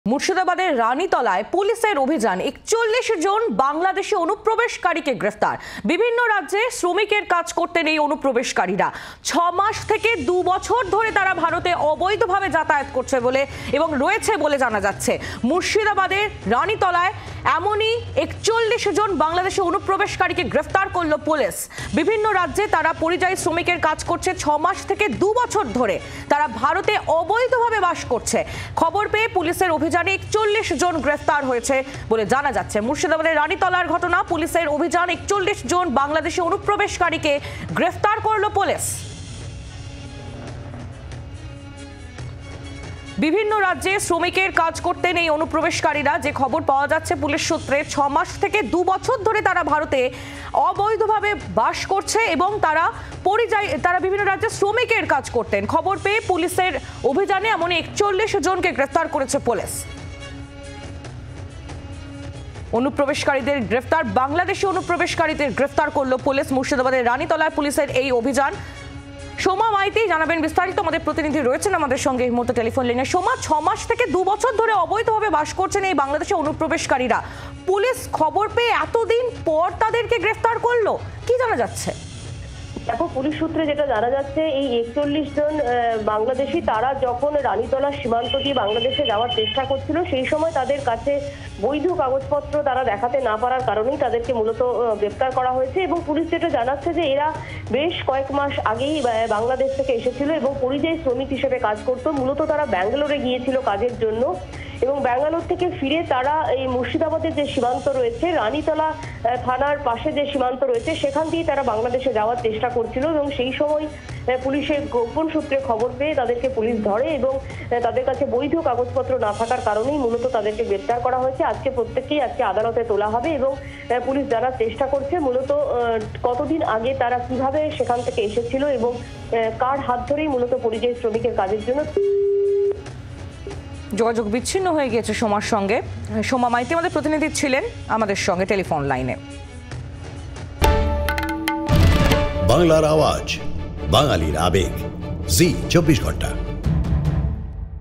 Bangladesh Issue | মুর্শিদাবাদে অভিযান, ৪১জন বাংলাদেশি অনুপ্রবেশকারীদের গ্রেফতার | Zee 24 Ghanta
Bangladesh Issue | মুর্শিদাবাদে রানিতলায় অভিযান, ৪১জন বাংলাদেশি অনুপ্রবেশকারীদের গ্রেফতার | Zee 24 Ghanta